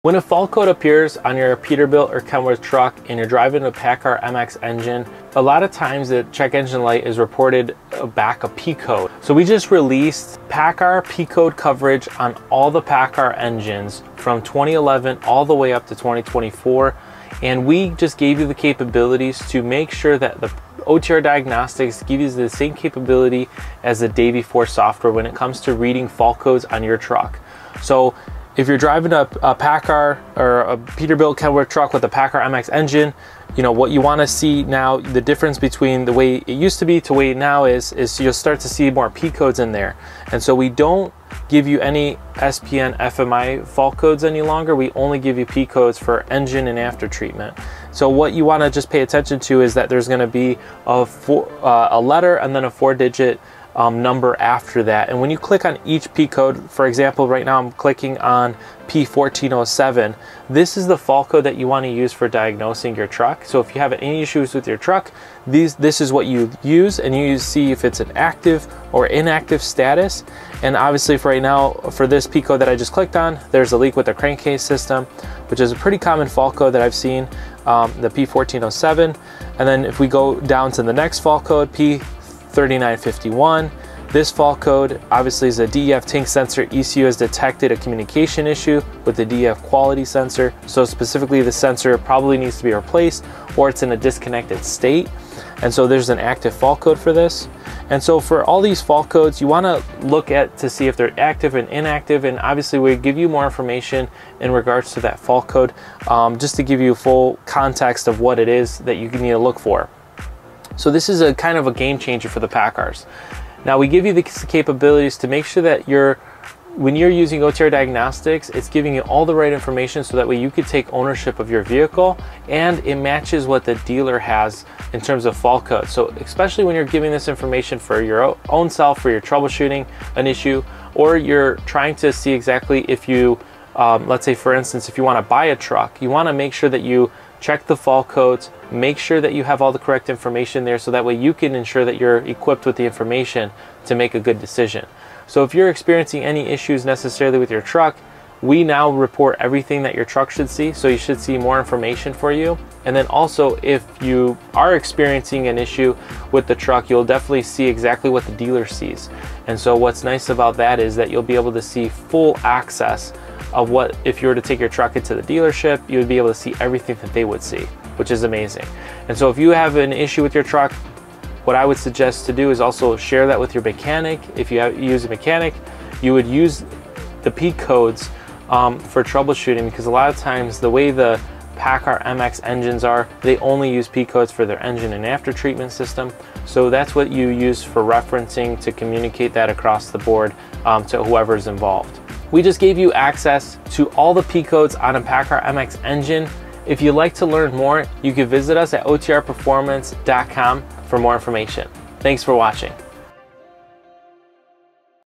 Paccar MX OEM P-codes capabilities, now in the palm of your hand | OTR Diagnostics
Want to learn more? Check this out

OTR Diagnostics offers a comprehensive solution to many heavy-duty truck problems of all makes and models. Here's how it works:

Detailed Fault Code Explanation: OTR Diagnostics provides an in-depth understanding of the SPN 5246 fault code, explaining its causes and implications.

Live Data Parameters: The tool offers real-time access to live data, allowing users to monitor engine performance and identify issues quickly.

Step-by-Step Repair Guidance: Users receive step-by-step instructions to diagnose and resolve the fault code, empowering them to perform repairs themselves.

Cloud Storage: All diagnostic data is stored in the cloud, making it accessible anytime and from anywhere, ensuring that users have all the information they need at their fingertips

All product and company names are trademarks™ or registered® trademarks of their respective holders.